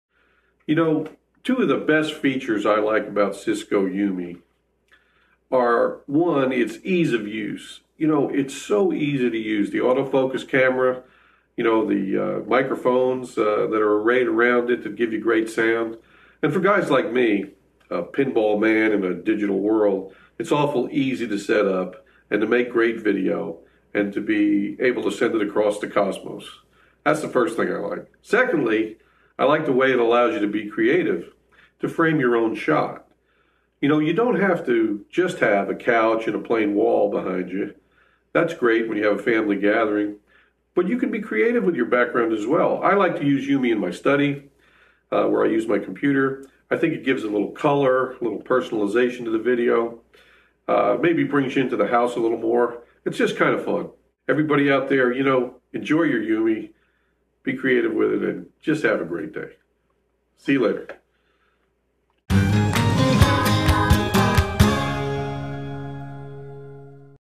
A Message from a Cisco ūmi User: Be Creative!
Check out this ūmi video message recorded straight from Jim's ūmi system and hear what his favorite features are!

Follow in Jim's footsteps and tell us what you love about Cisco ūmi

We'll select the best video messages and post them on our Facebook and Twitter pages, as well as our blog, in order to showcase how people are using Cisco ūmi to connect with loved ones across the nation.